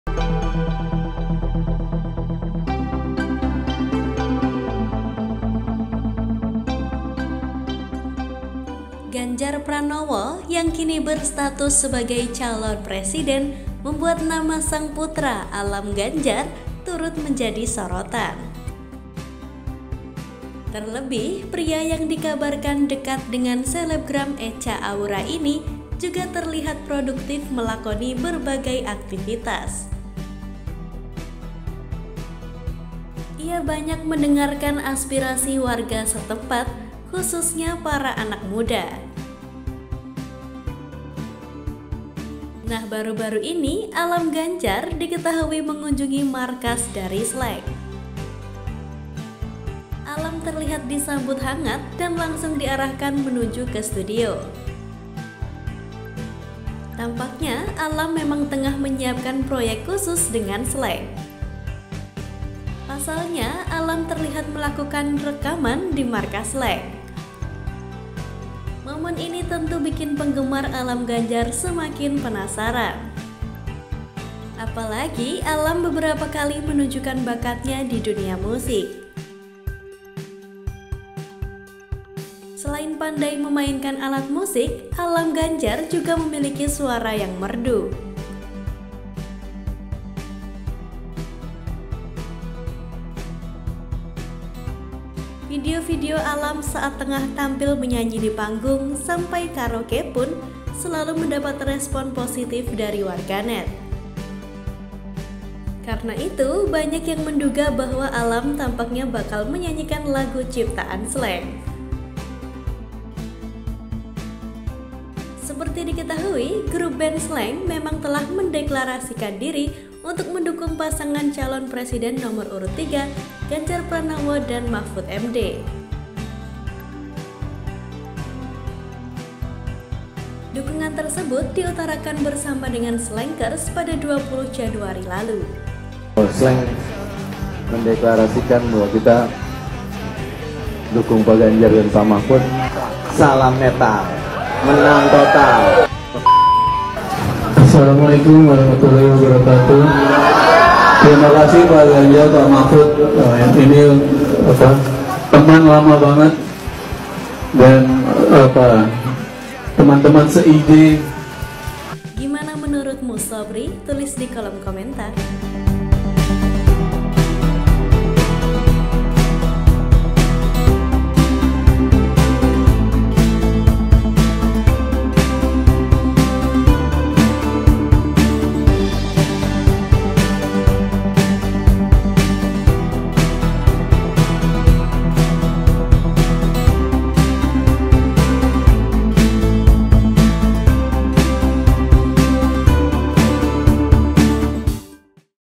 Momen Alam Ganjar kunjungi markas Slank, langsung coba rekaman
Koordinator: Agib Tanjung
Editor Grafis: Restu Anggoro | Fathur Rahman H